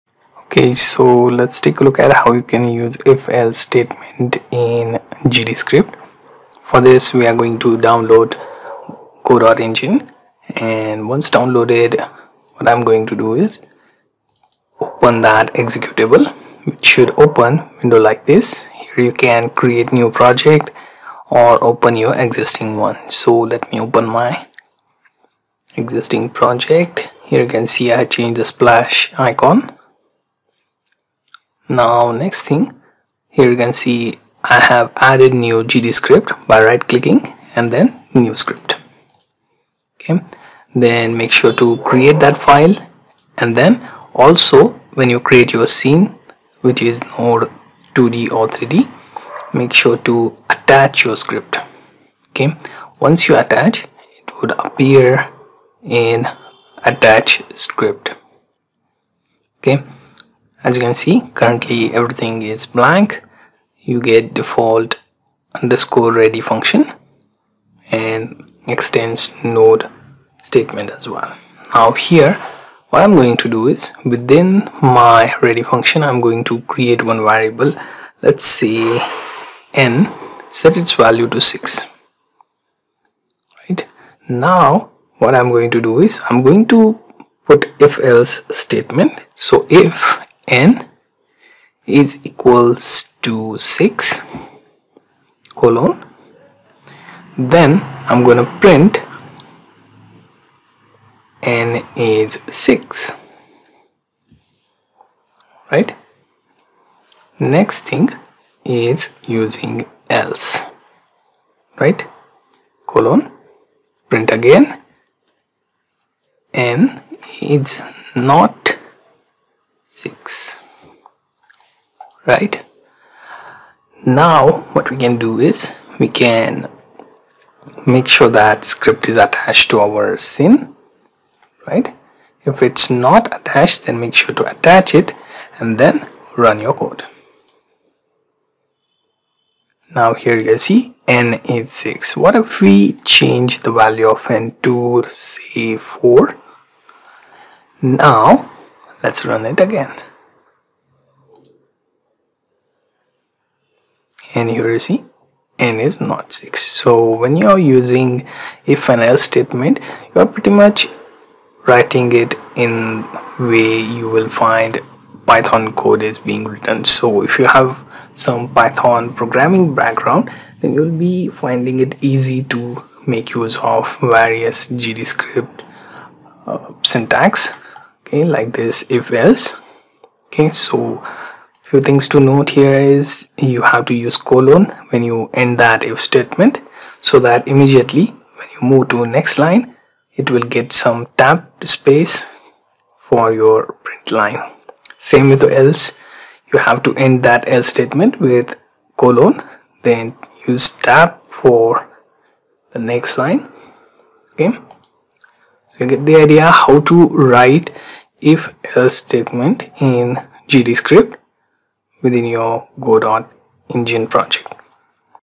Godot Engine GDScript - How to Use If else statement
Learn How to Use If else statement in GDScript in Godot Engine.